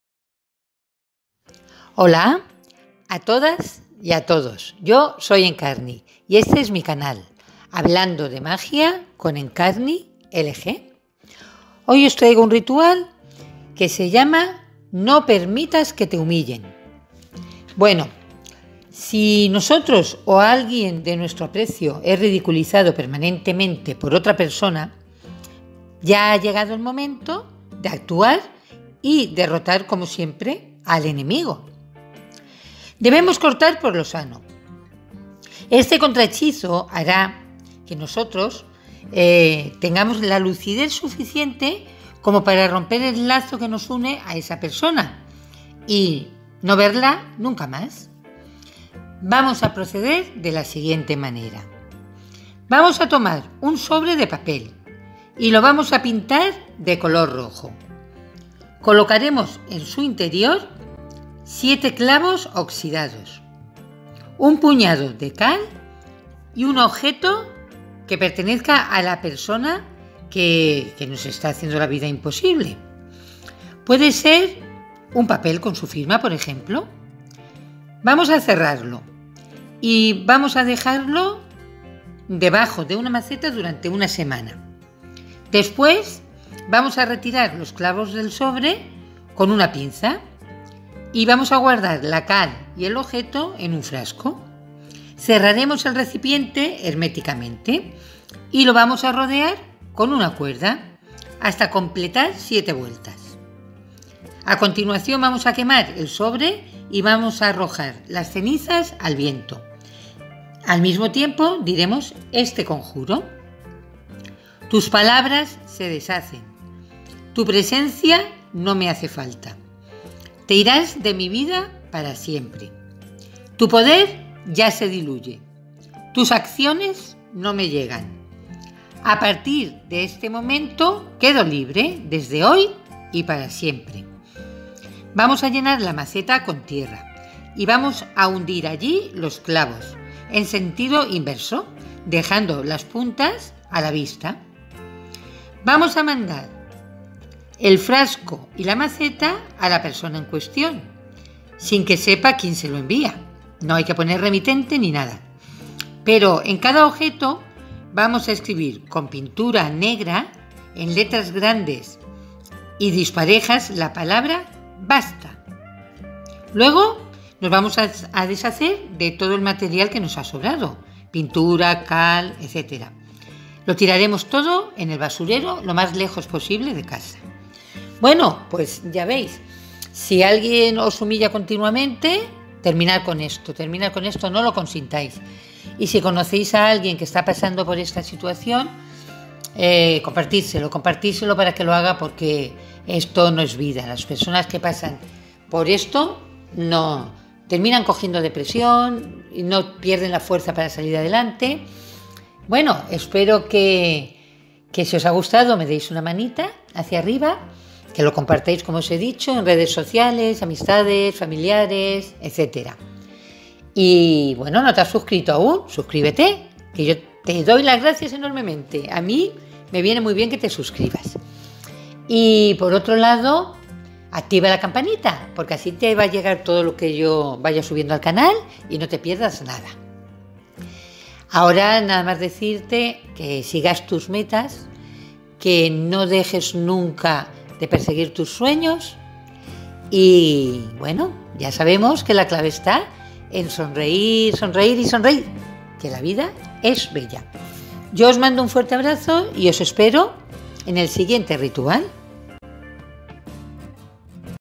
NO PERMITAS QUE TE HUMILLEN
Si alguien tiene por mala costumbre de ridiculizarte, humillarte, este hechizo te protegerá y devolverá el mal hecho.